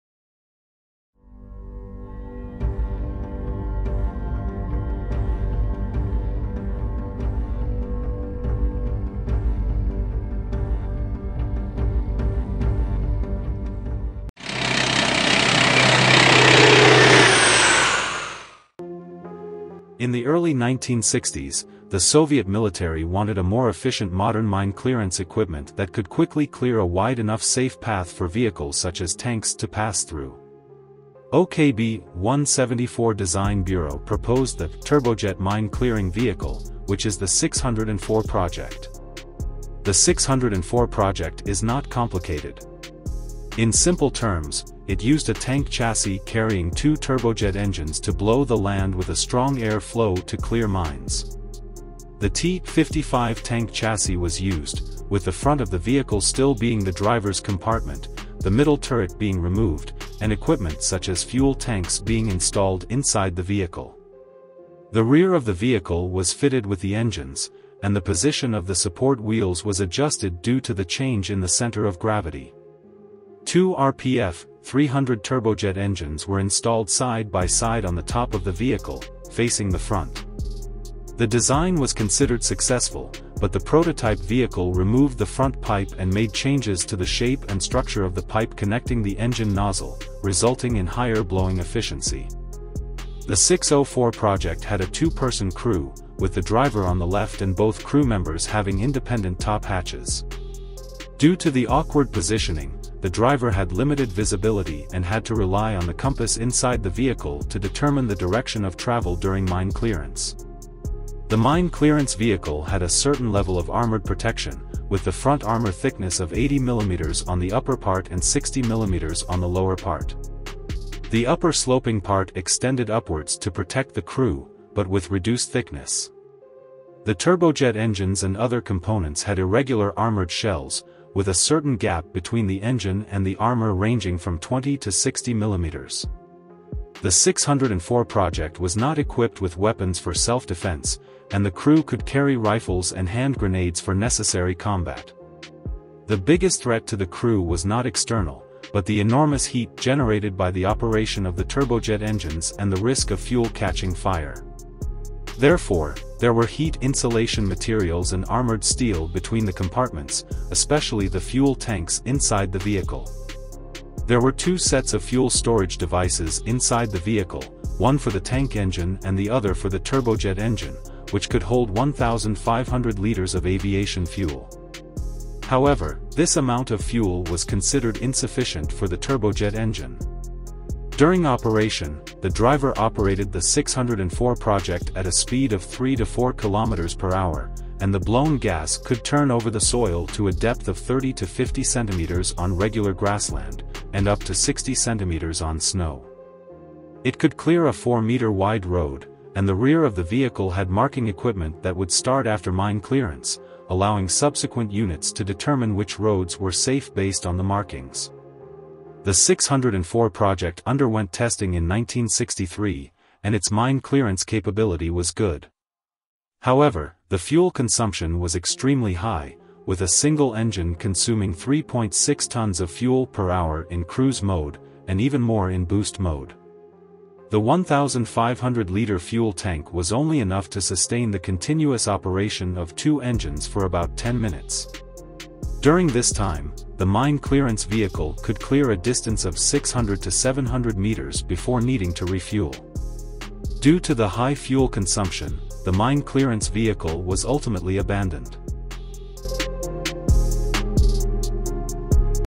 Jet-Powered Mine Clearing Tank: The 604 Project
The 604 project was a Soviet mine clearance vehicle using a tank chassis and two turbojet engines to blow the land with a strong air flow to clear mines, but it was ultimately abandoned due to high fuel consumption. The vehicle had a 2-person crew, limited visibility for the driver, and a certain level of armored protection, but was not equipped with weapons for self-defense.